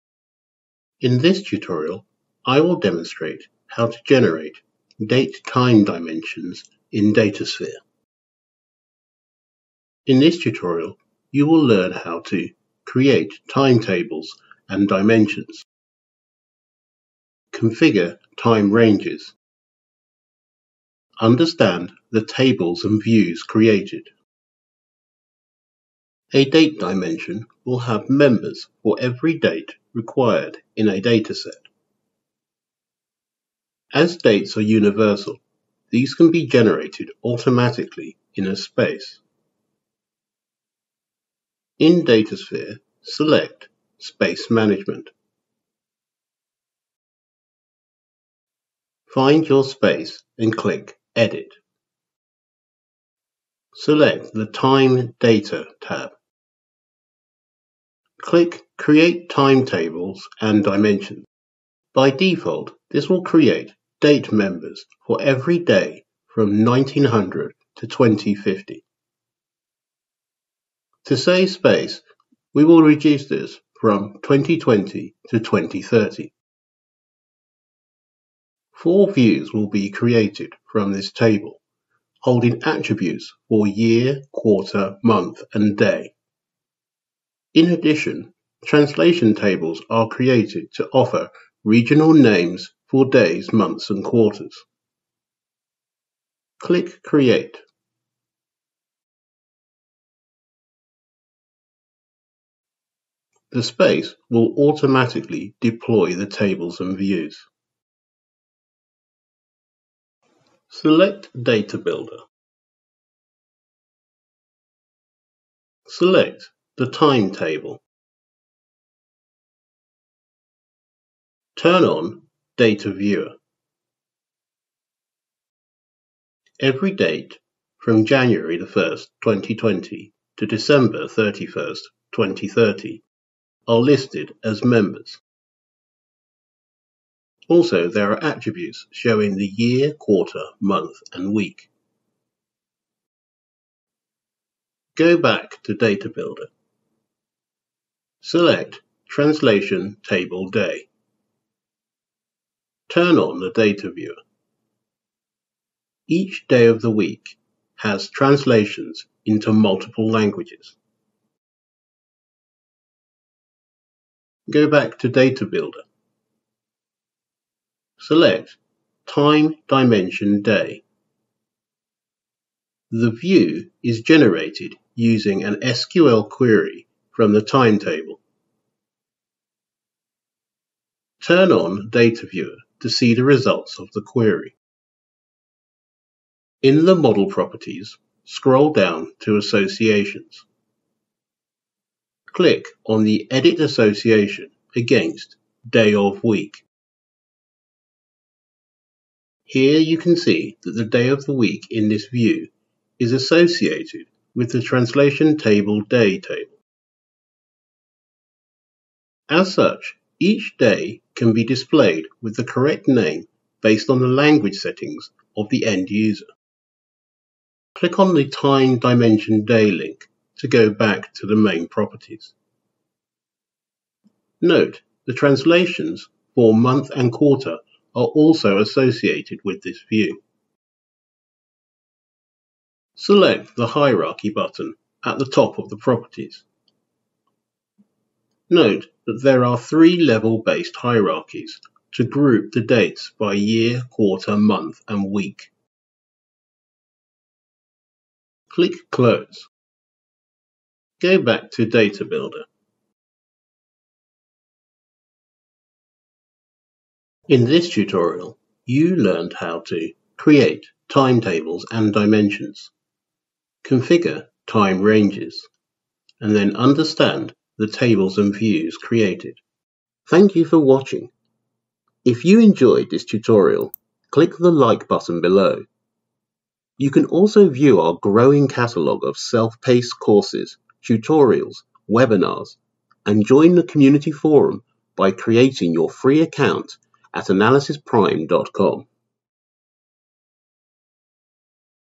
Automatic Date - Time Dimension in SAP Datasphere
In this tutorial, our instructor will demonstrate how to add an automatic Date/Time dimension to a space in SAP Datasphere. You will learn how to create Time tables and Dimensions, configure time ranges, and understand the tables and views that have been created.